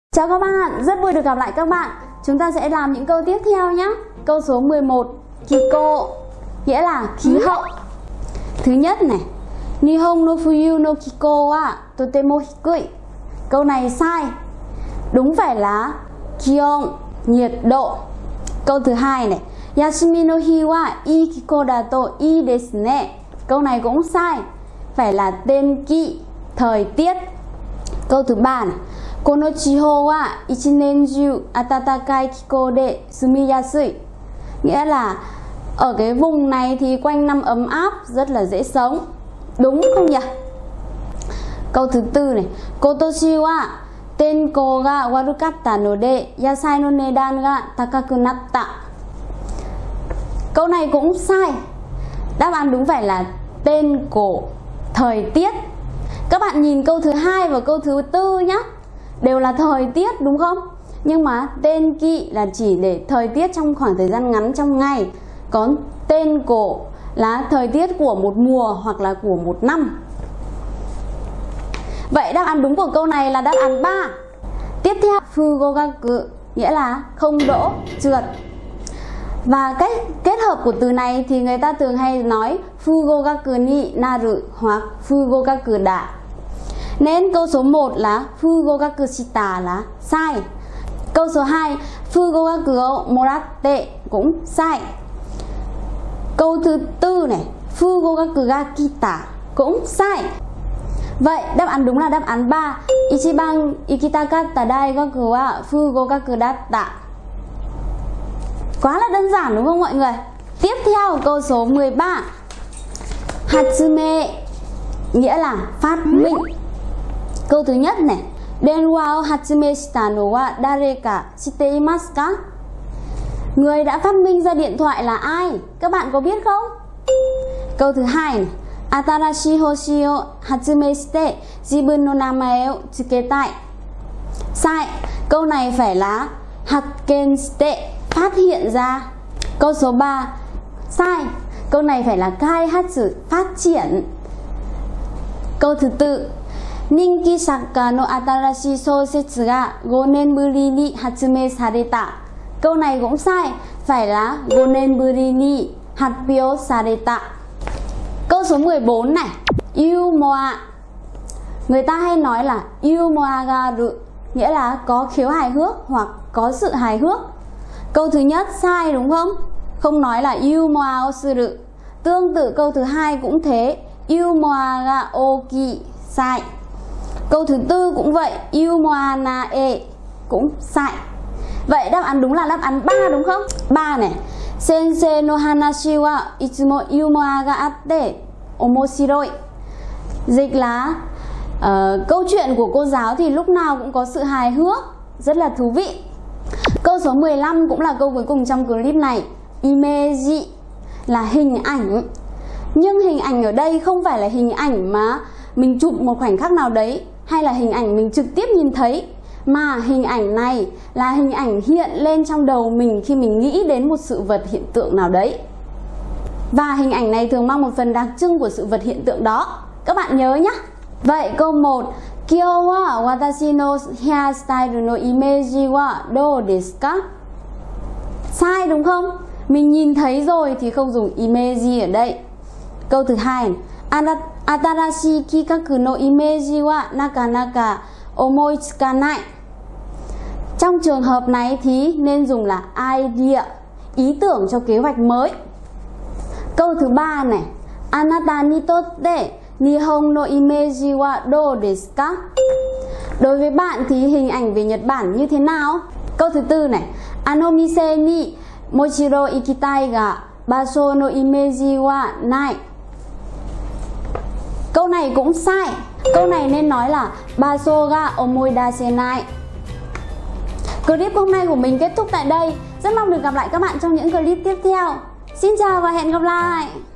#3 Chữa đề luyện thi N3 - phần từ vựng
Luyện thi N3 Nihongonomori
Chữa đề luyện thi N3 - phần từ vựng
1. 気候 - khí hậu
2.不合格 - trượt, không đỗ
3.発明 - phát minh
4.ユーモア - có khiếu hài hước, sự hài hước
5.イメージ - hình ảnh